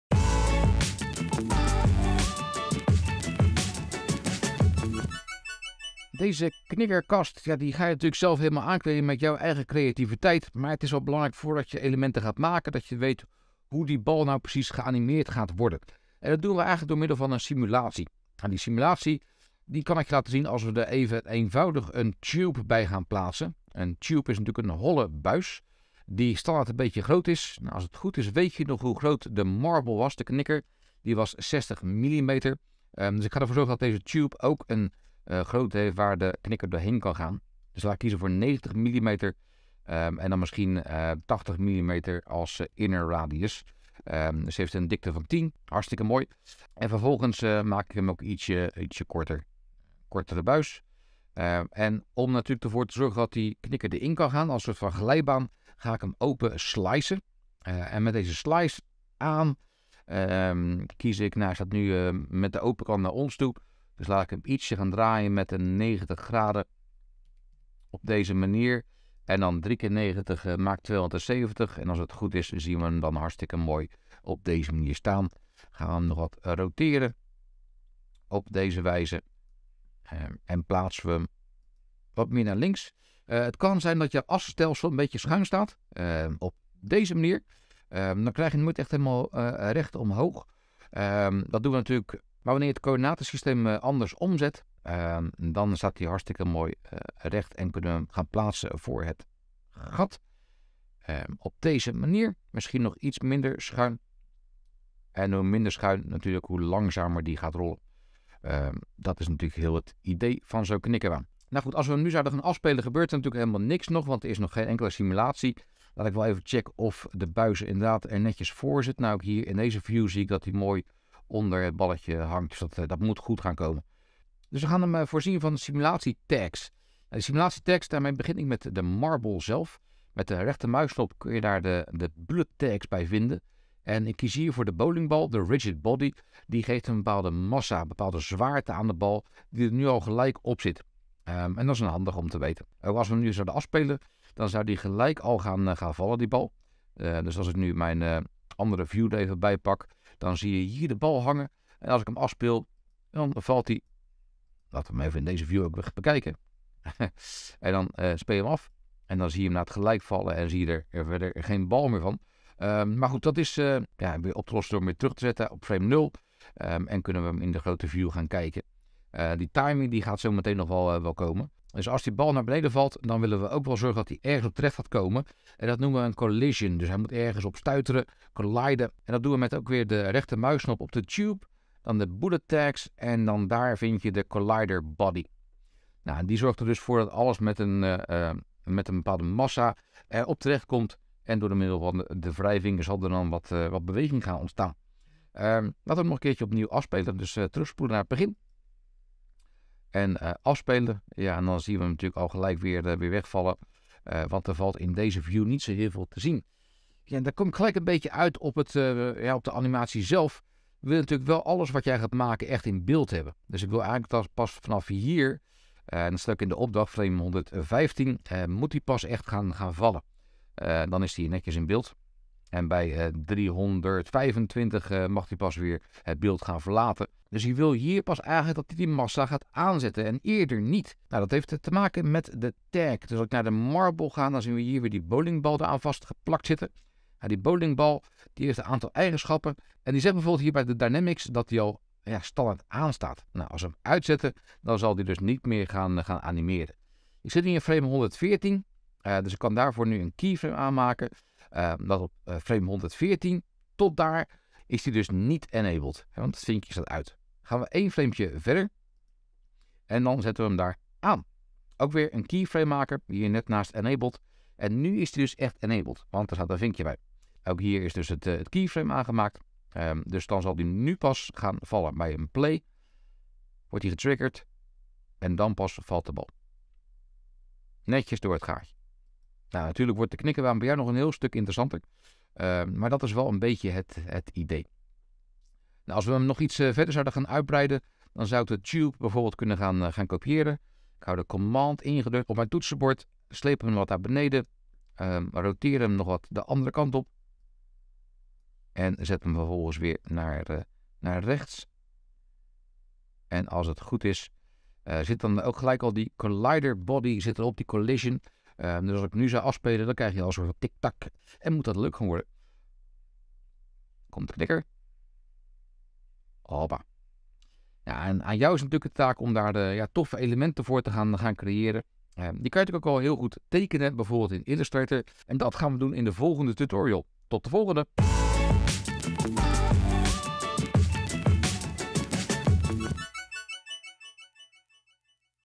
SOFTAN | Marble Maze | 02 | Animeren met simulatie?!?
In Cinema4D kan je het animeren van een object ook laten doen door simulatie. Het komt er simpel weg op neer dat je een object een bepaald gewicht geeft en een ander object moet dan een object zijn waartegen het moet botsen. Als het zware object daarop valt, stopt het of rolt het weg in het geval van de knikkerbaan. Je gaat daarom werken met collision objects en een rigid body.

Dit is heel erg basis nog. Wel een basis die je moet snappen voordat je objecten gaat bouwen in jouw knikkerkast.